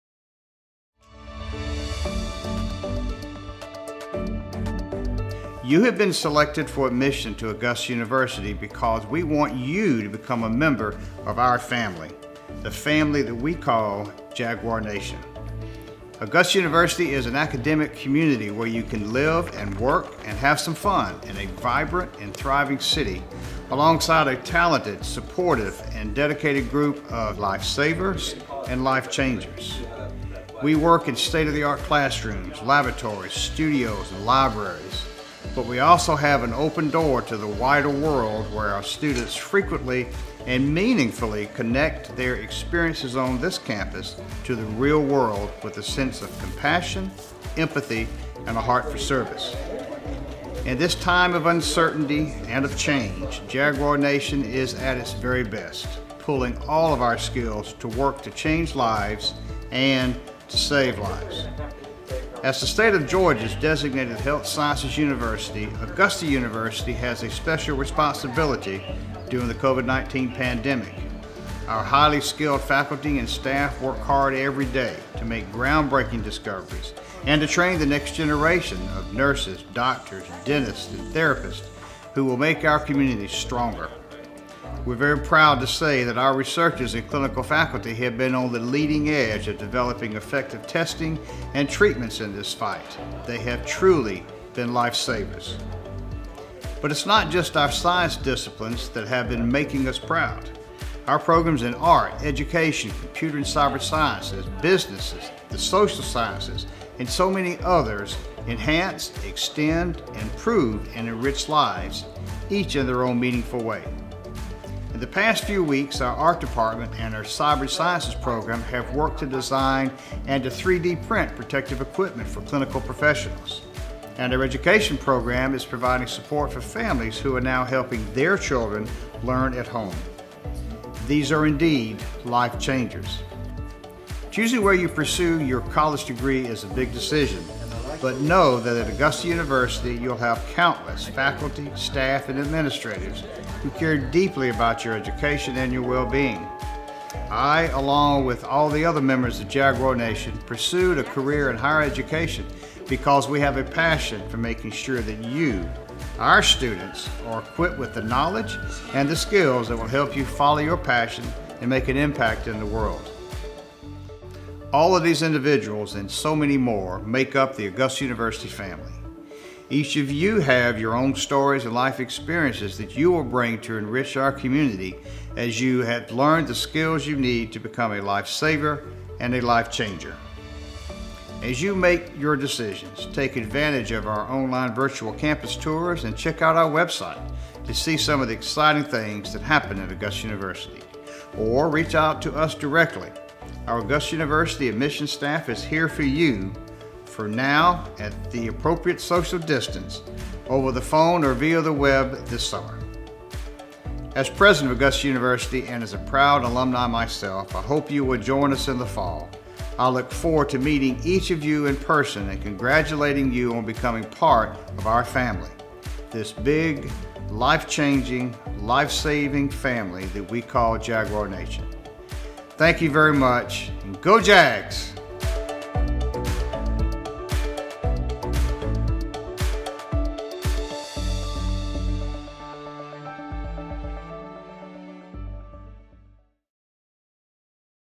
Welcome To Jaguar Nation
Congratulations on your admission to Augusta University! You have been selected for admission to Augusta University because we want you to become a member of our family: the family we call Jaguar Nation!

Augusta University is an academic community where you can live and work and have some fun in a vibrant and thriving city, alongside a talented, supportive and dedicated group of life savers and life changers. We work in state-of-the-art classrooms, laboratories, studios and libraries. But we also have an open door to the wider world where our students frequently and meaningfully connect their experiences on campus to the real world with a sense of compassion, empathy and a heart for service.

Choosing where you pursue your college degree is a big decision: but know that at Augusta University, you’ll have countless faculty, staff, and administrators who care deeply about your education and your well being. We're passionate about making sure our students are equipped with the knowledge and skills that will help them follow their passion and make an impact in the world.

This video was produced and created by the corporate video team in the Division of Communications & Marketing at Augusta University.